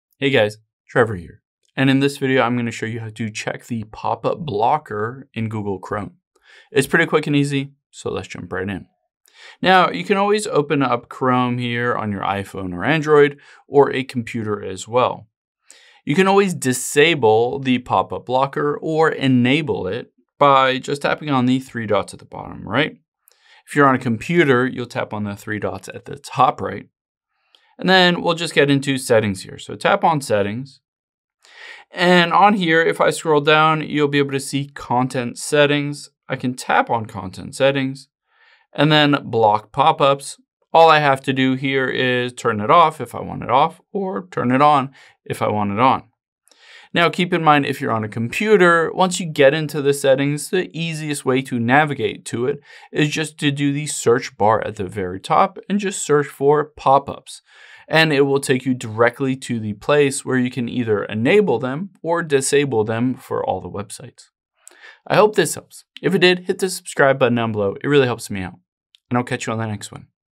How To Check Pop Up Blocker On Google Chrome
Here's how to check whether your Google Chrome pop up blocker is turned on or off and change it if you want.